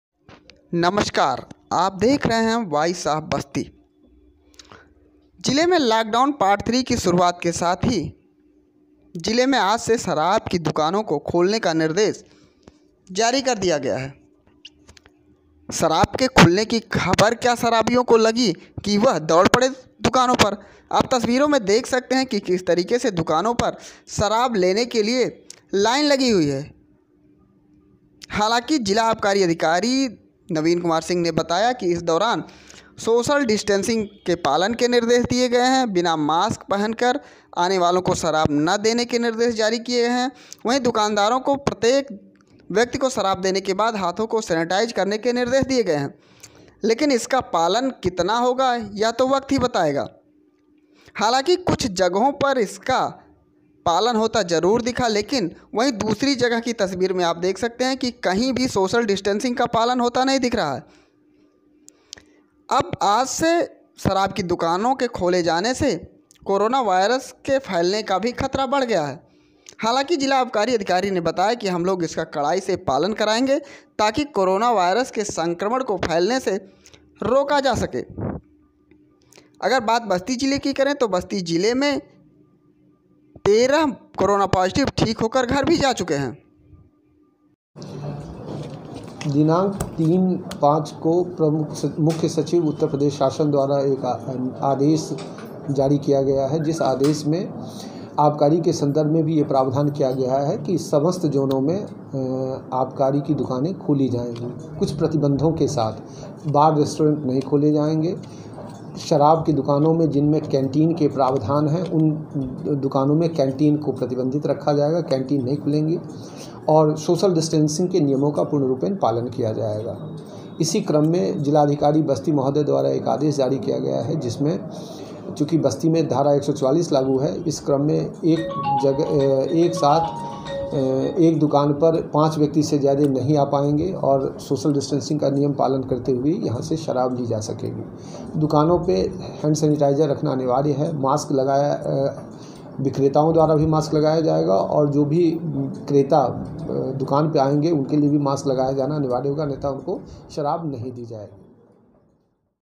लॉक डाउन पार्ट 3 की शुरुआत के साथ खुली शराब की दुकानें, बिना मास्क के नही मिलेगी शराब
आज से लॉक डाउन  पार्ट 3 की शुरुआत हो गई है। शासन स्तर से तमाम दुकानों को खोलने की छूट भी जारी की गई है। बस्ती जनपद में भी तमाम दुकानें आज से खुल गई हैं। जनपद में शराब की सभी दुकानें 10 बजे से 2 बजे तक खोलने का जिलाधिकारी ने  आदेश जारी किया है। जिला आबकारी अधिकारी नवीन सिंह ने बताया कि शराब की जो भी दुकानें खोली गई हैं। उन्हें सख्त निर्देश दिया गया है कि सोशल डिस्टेंसिंग का पालन प्रत्येक हाल में करना है। ऐसा न करने वाले दुकानदारों के ऊपर जिला प्रशासन की तरफ से कार्रवाई भी की जाएगी। आबकारी अधिकारी ने बताया कि साथ ही हैंड सैनिटाइजर का भी उपयोग निरंतर करते रहना है जिससे संक्रमण न बढ़ने पाए।